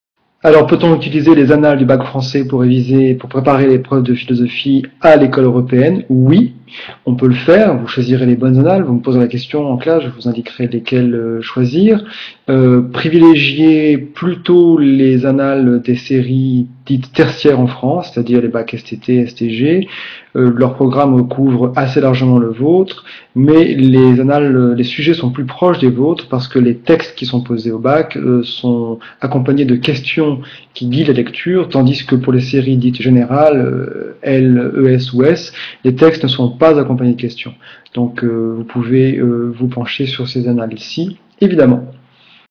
Peut-on utiliser les annales du bac français pour préparer l'examen de philo ?
Conseil pour les élèves de l'école européenne.